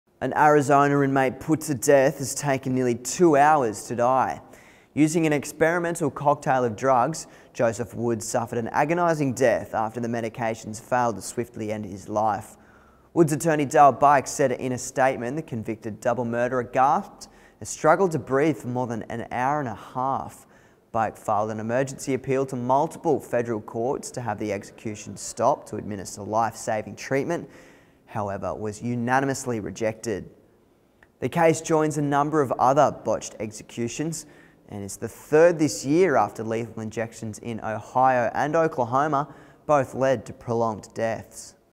Man Takes Two Hours To Die In Botched Execution - July 25, 2014
Using an experimental cocktail of drugs, Joseph Wood suffered an agonising death after the medications failed to swiftly end his life.